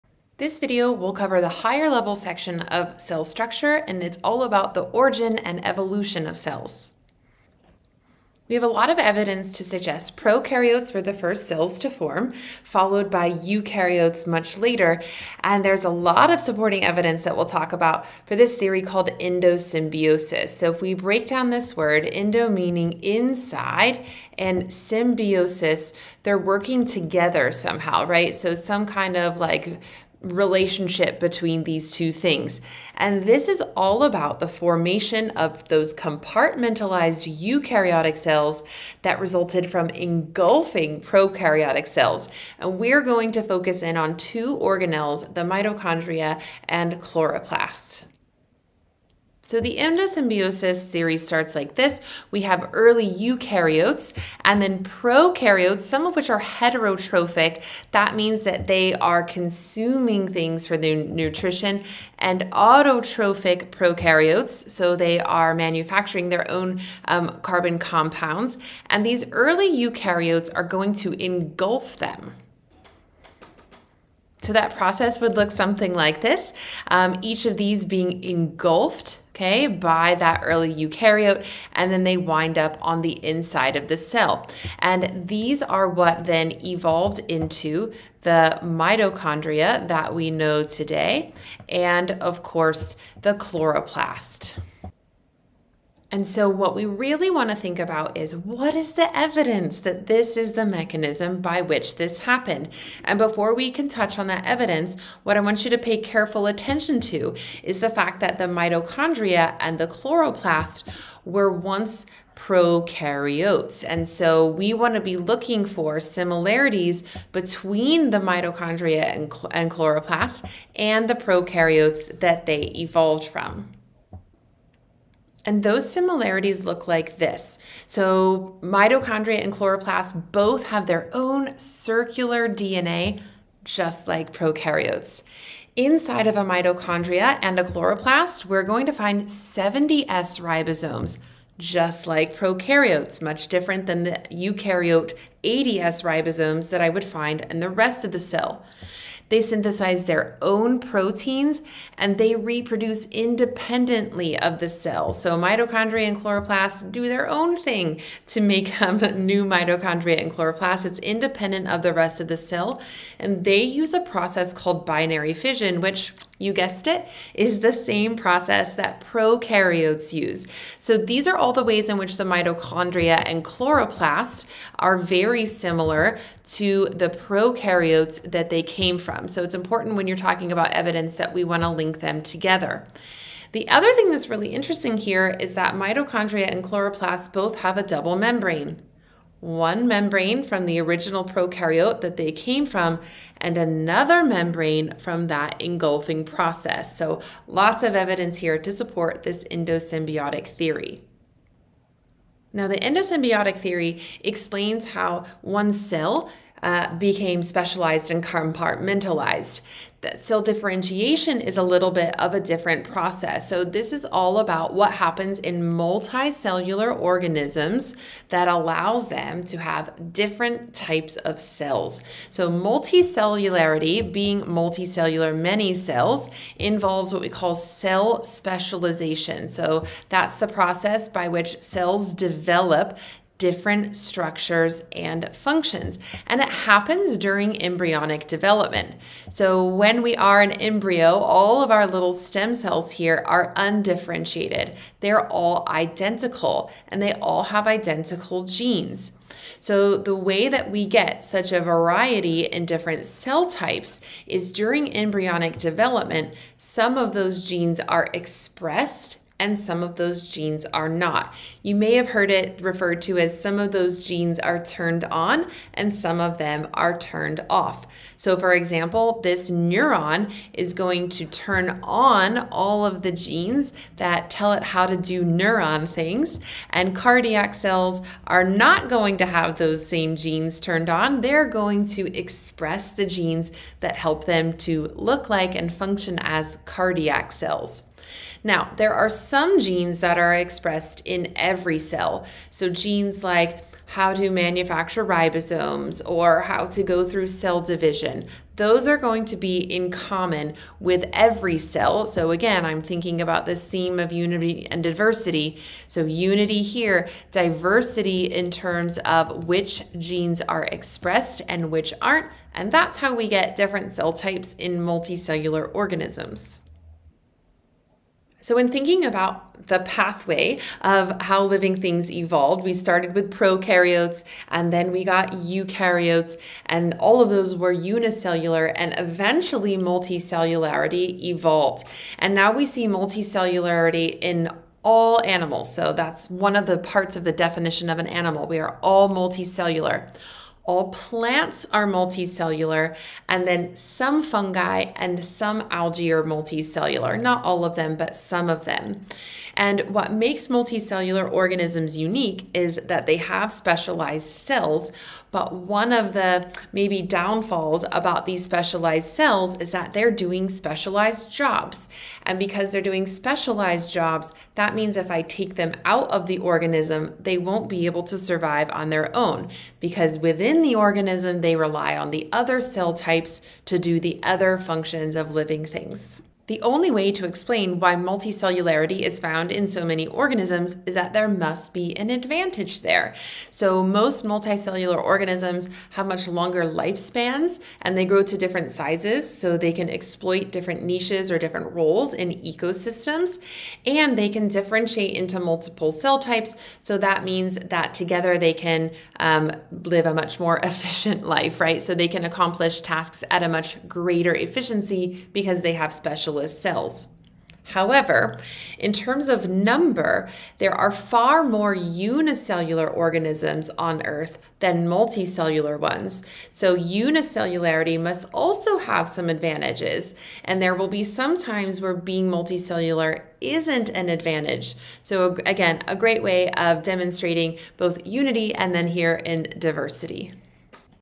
A2.2 HL Origin and Evolution of Cells [IB Biology HL]
This is part of the IB's NEW Biology syllabus, which means you have exams in May 2025 or later. The videos were designed and recorded by Cheryl Hickman